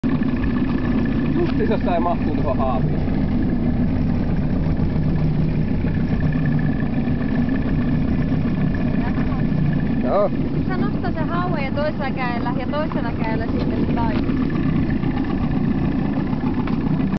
Oulujärvi - Haukea nousee 2009
Lisää oulujärven kalastelua, päivän hauki saatiin juuri ylös